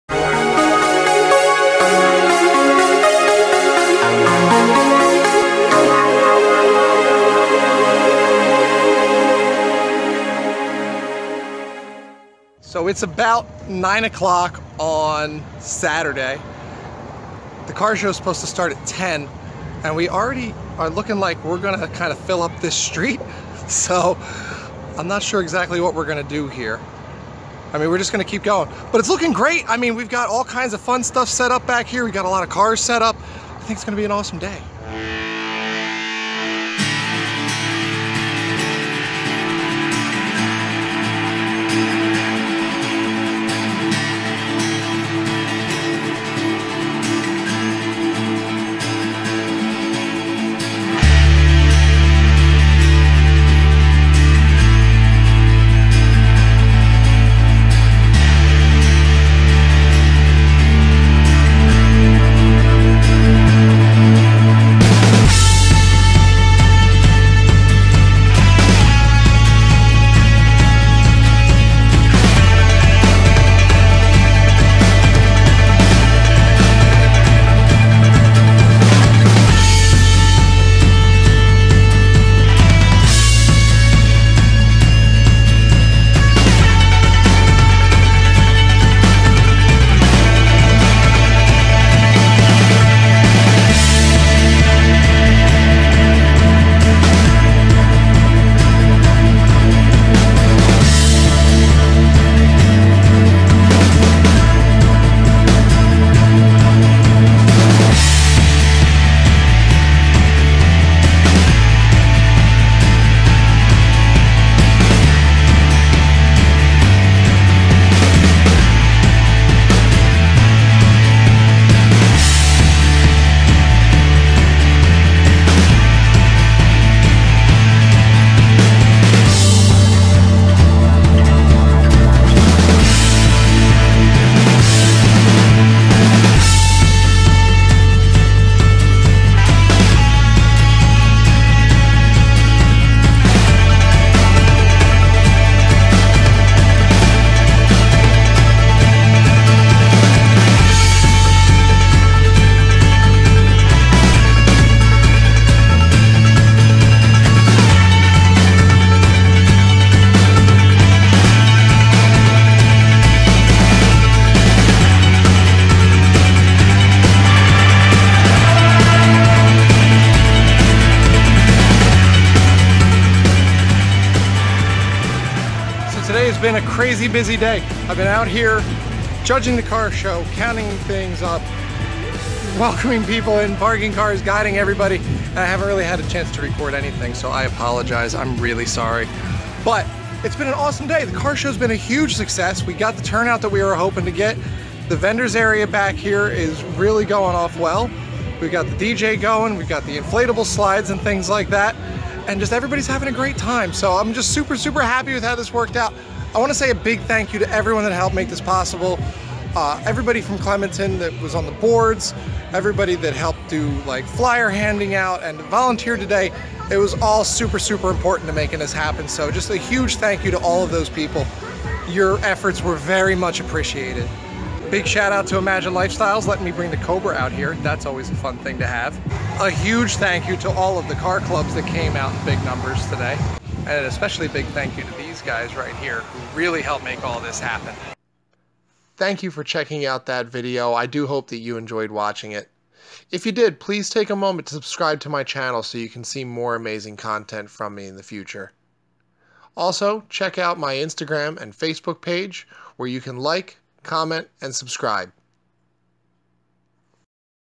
Clementon Fall Fest & Car Show 2017
Thank you for joining us for the Clementon Fall Fast Car, Truck, and Bike show 2017. This was our inaugural year for this event, with plans for us to bring it back bigger and better next year!